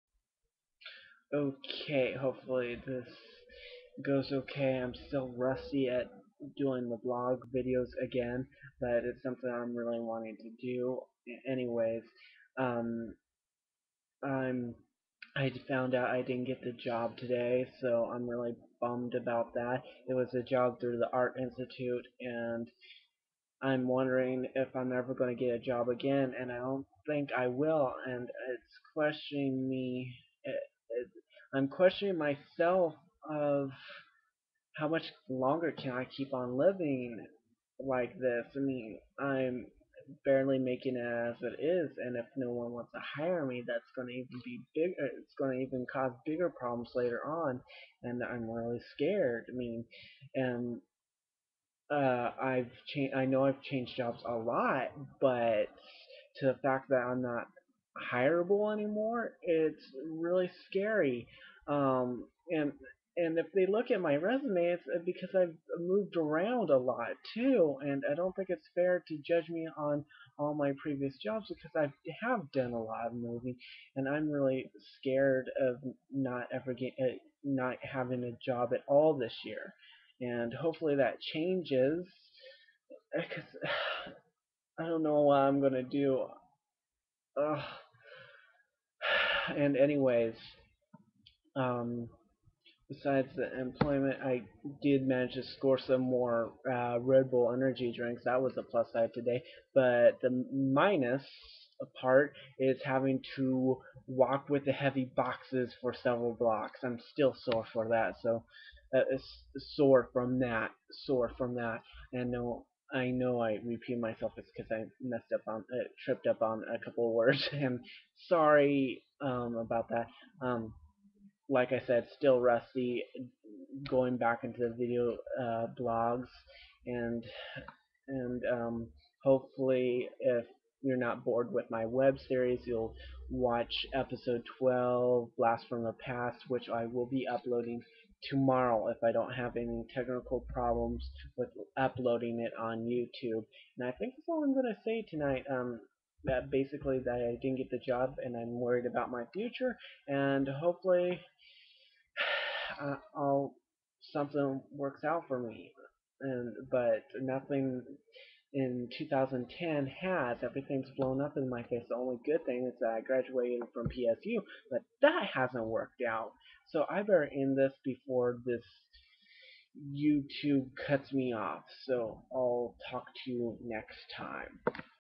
Video Making Freak: Still Jobless College Grad
Feeling blue of finding out I did not get the job, and plan to go into more detail with part two
Note: I know the audio is not the best, but do not have the money to update my equipment sorry. If you want to leave a comment do so on my channel
UPDATE 11/19/2010: THERE WILL BE NO PART TWO BECAUSE MY COMPUTER CRASHED. I AM USING THE SCHOOL COMPUTERS TO CHECK MY YOU TUBE ACCOUNT